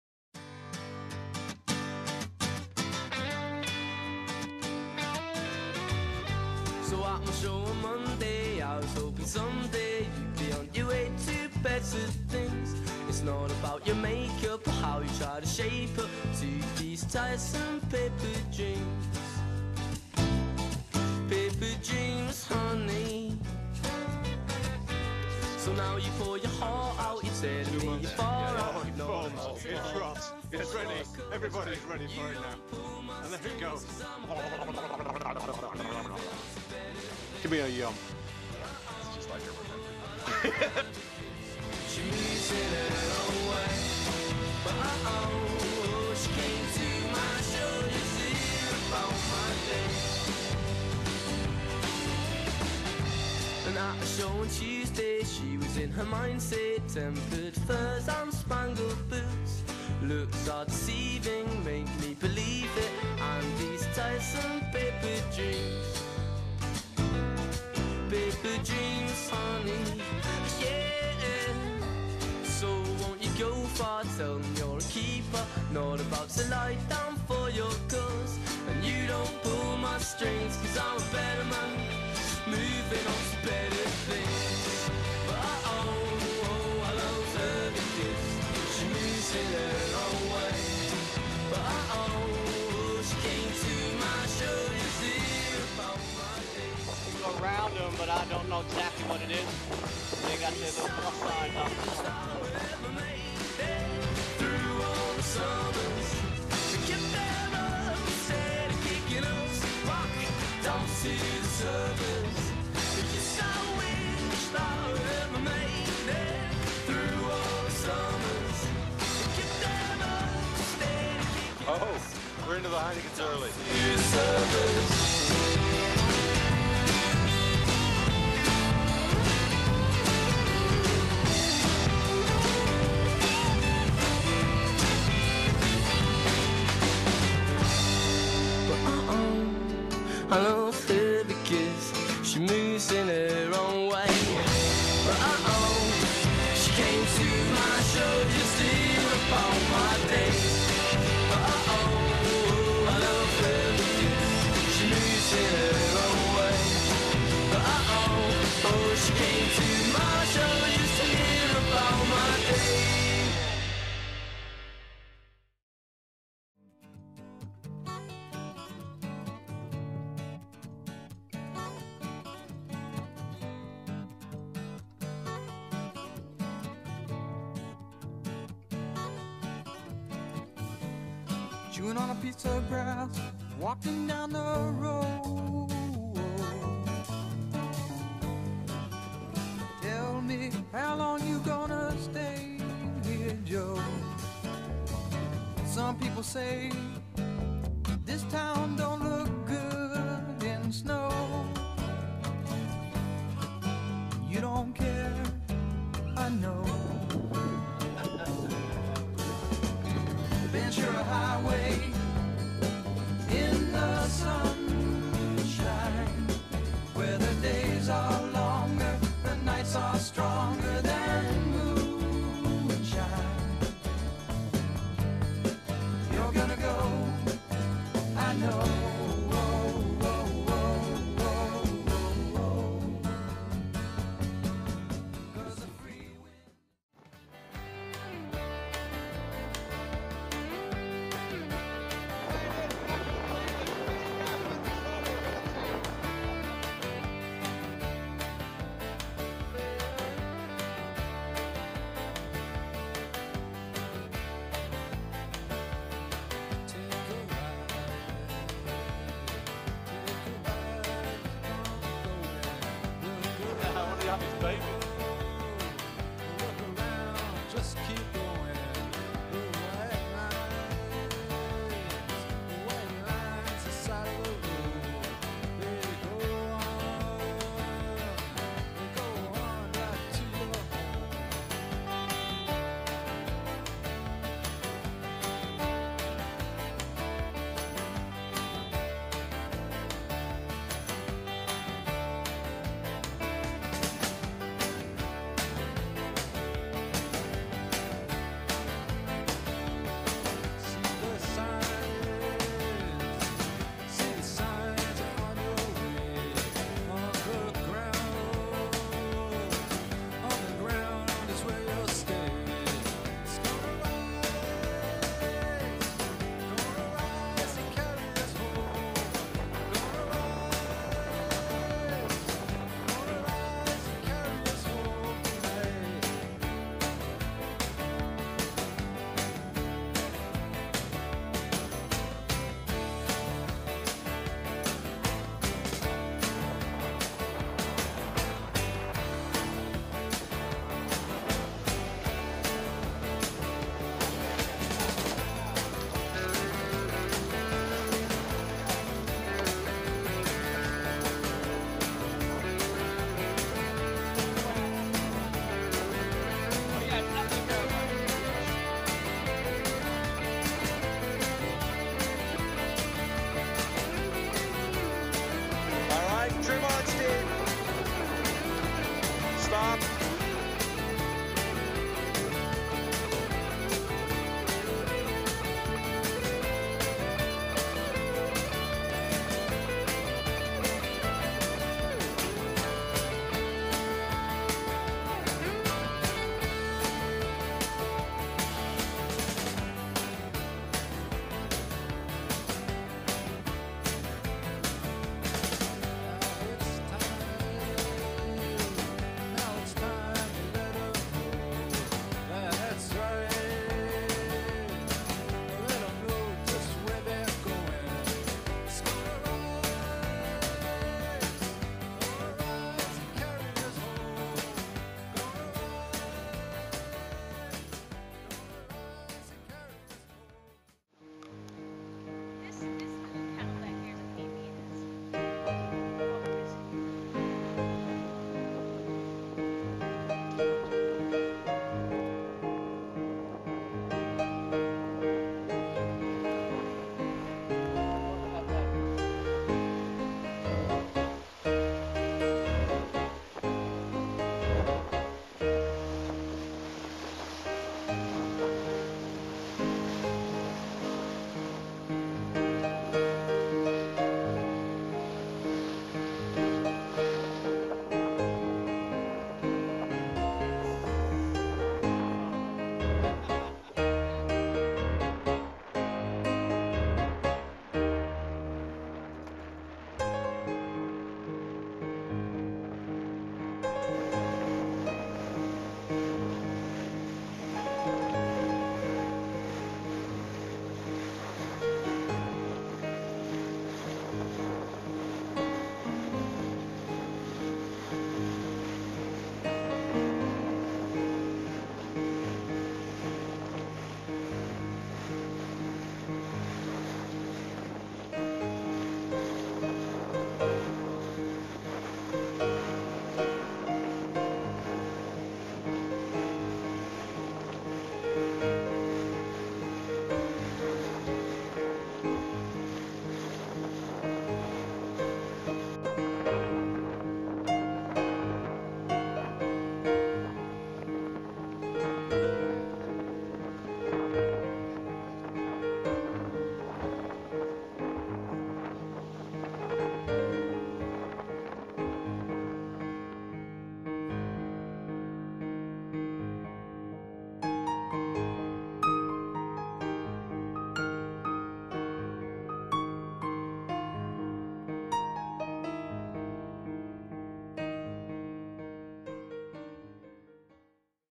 St. Maarten Heineken Regatta 2010 on Big Bear
It was a great time sailing in the 2010 St. Maarten Heineken Regatta on Jon Brewin's J160 "Big Bear". We were the smallest boat in the Spinnaker 2 class but we had the biggest mothership, Watercolours...a really nice combination! Many thanks to Bill and Maggie and a great crew. Sail with good friends. Life is too short to put up with morons or drink bad wine.